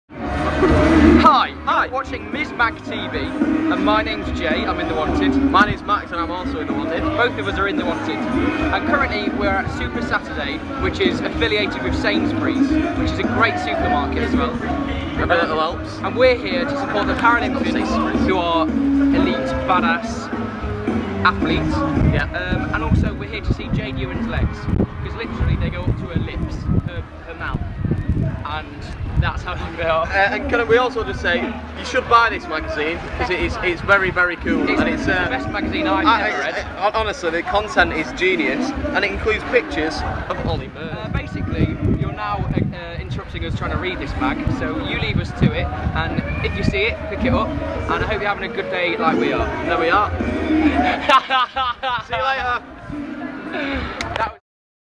The Wanted chat to Mizz at Sainsbury's Super Saturday
The Wanted chat to Mizz at Sainsbury's Super Saturday. Jay and Max are loving Mizz, almost as much as we ♥ them!